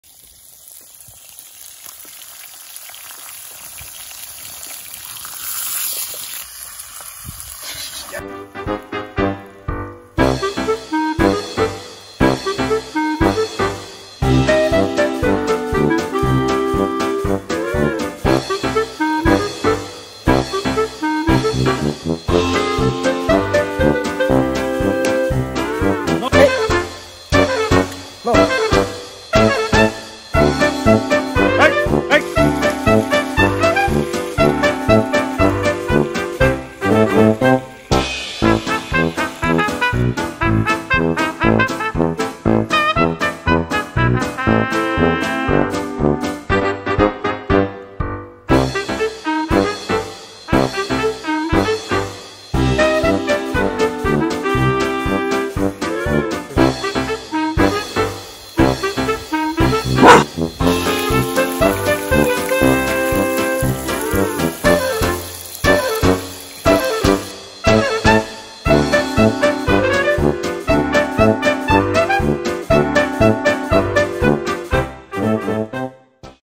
Dog begging for steak
Hungry rhodesian Ridgeback dog waiting for a steak and egg breakfast